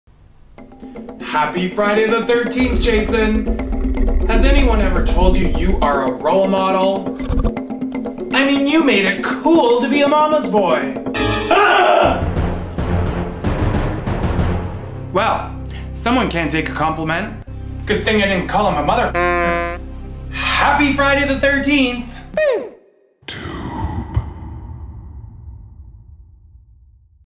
Don't Call Jason A Mama's Boy: Happy Friday The 13th
Ghost tries to wish Jason a happy Friday The 13th